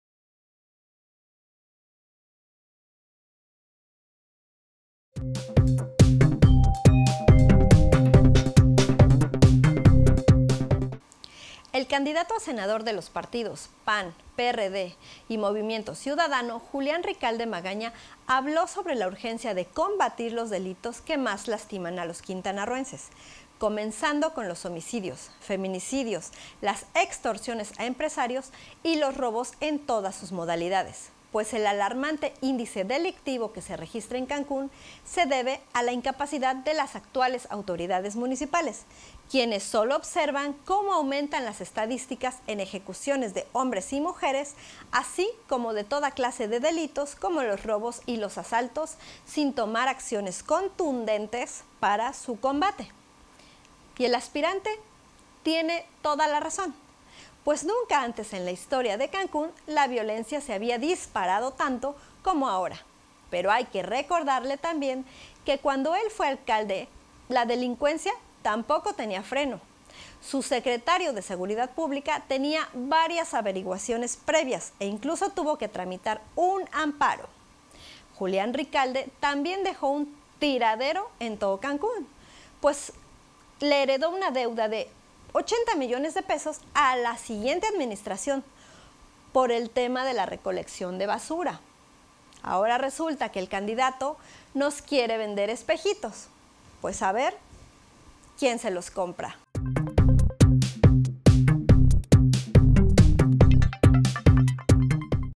JULIÁN RICALDE, CANDIDATO A SENADOR, QUIERE VENDER ESPEJITOS A QUINTANARROENSES
Entérate de las noticias “EN CORTO”…JULIÁN RICALDE, CANDIDATO A SENADOR, QUIERE VENDER ESPEJITOS A QUINTANARROENSES...